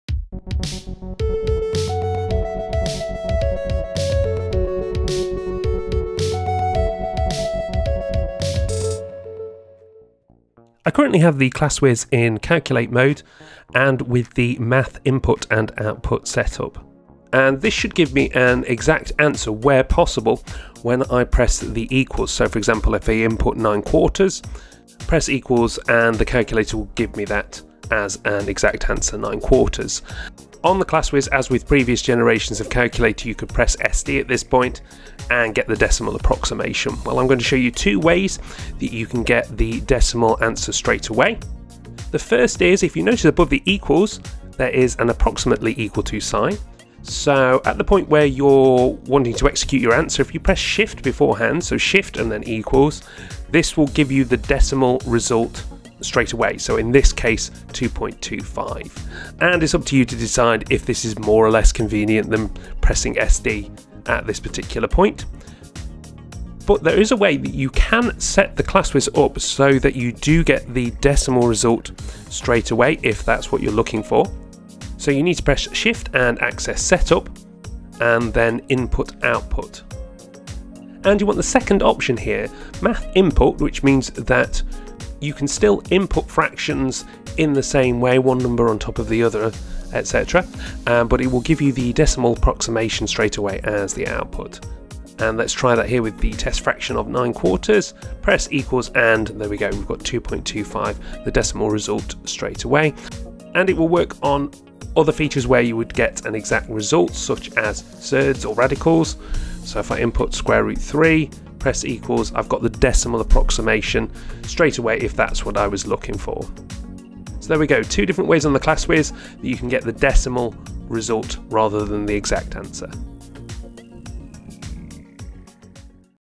Two Ways To Change To A Decimal Answer On A Casio Classwiz fx-991EX | fx-570EX Calculator
We know that we can press S=D to change an exact result to a decimal answer, but on the Classwiz there are two ways that can give you a decimal approximation. There is the approximately equal to feature and you can change the setting so that you get a decimal result every time.